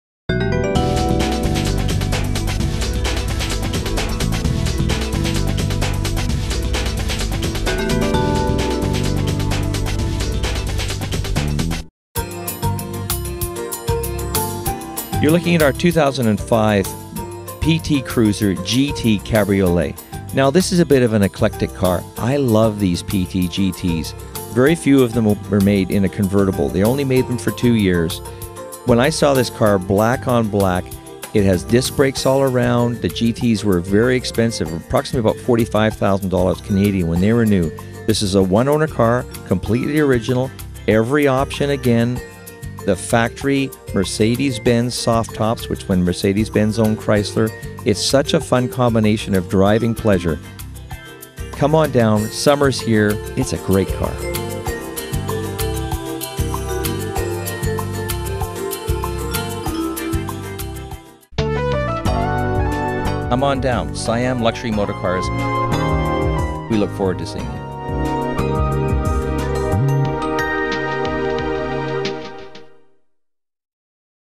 2005 Chrysler PT Cruiser GT Convertible
2005 Chrysler PT Cruiser GT Convertible. Black exterior on Black leather interior. Very LOW 53,000 km. One owner. for more details.